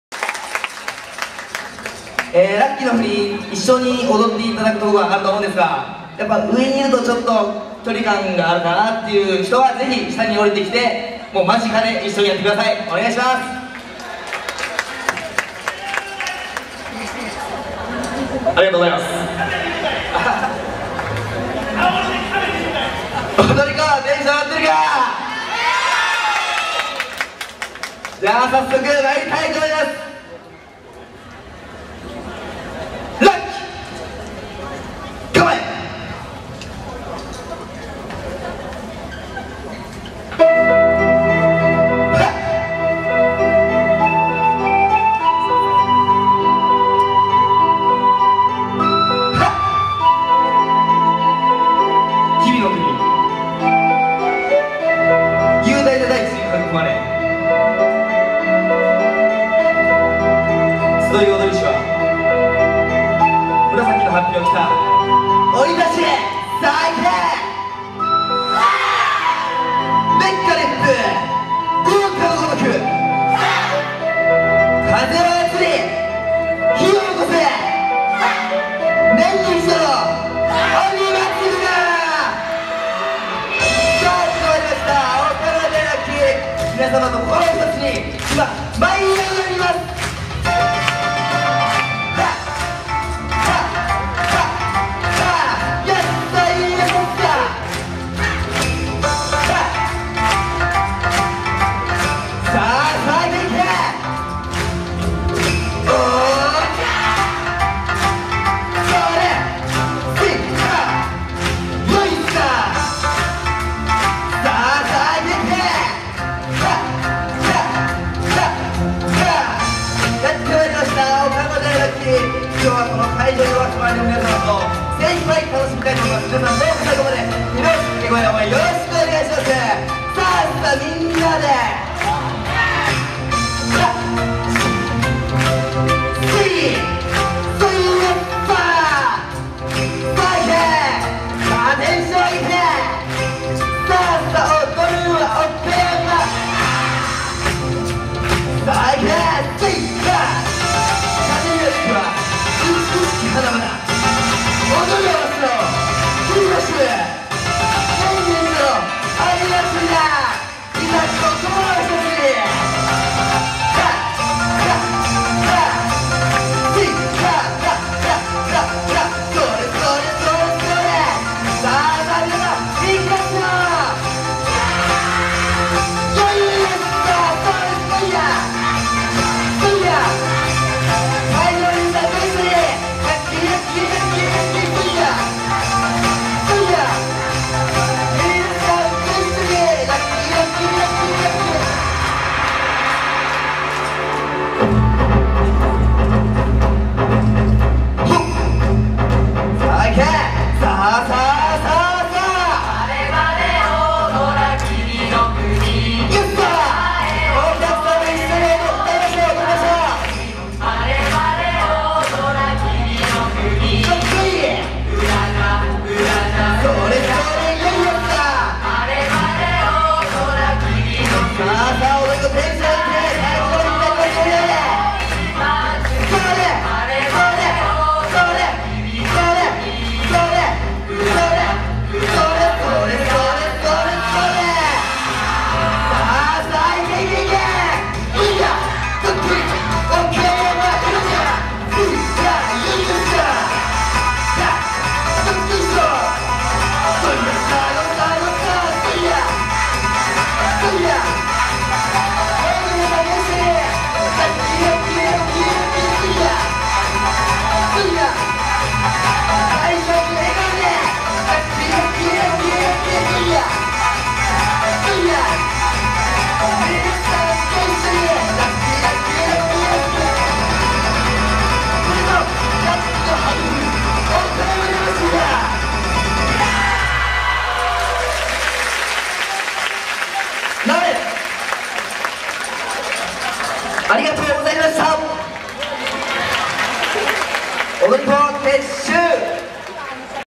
楽鬼〜優〜＠岡山武道館08(8/24)
8番目の踊り連は「楽鬼〜優〜」さんです。
(ピンボケですがご容赦ください)

2008,09,23(土)